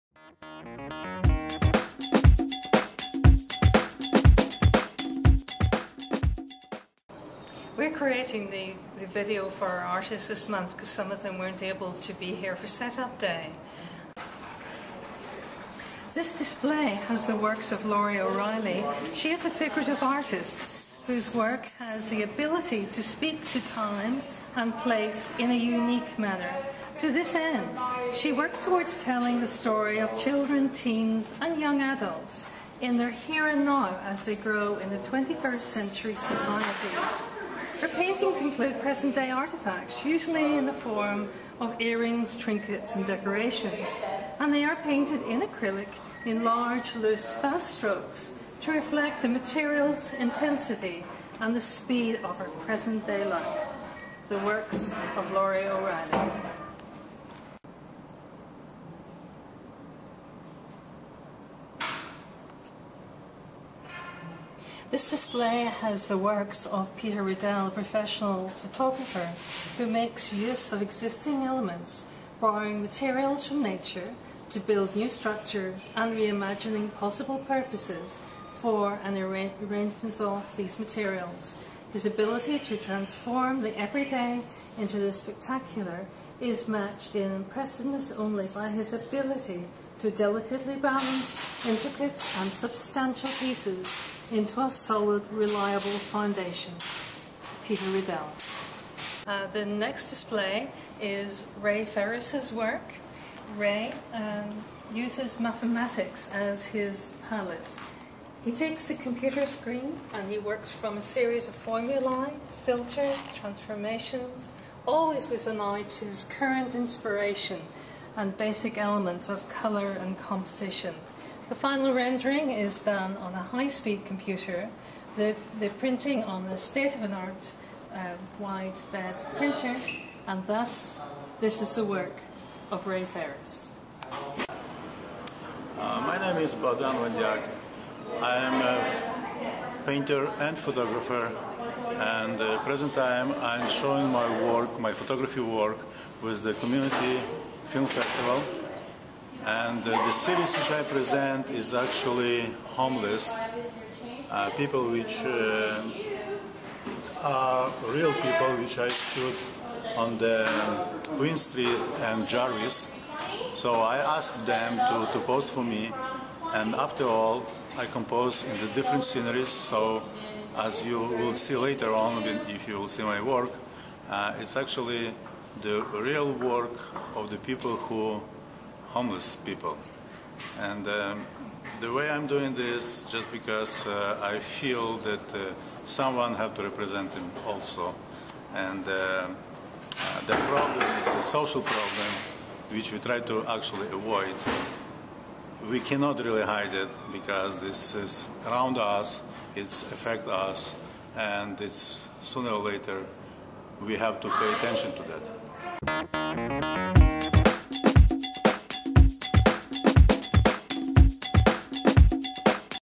September 2011 at Rainbow Gallery.wmv
Sheila J Mitchell, Executive Director of Art For All Canada,  introduces you to the art works of Laurie O'Reilly, Peter Riedel, Ray Ferris and Bohdan Vandiak (appearing in cooperation with COMMFFEST Community Film Festival) which are on display during September 2011 at the Rainbow Gallery, in downtown Toronto, Canada.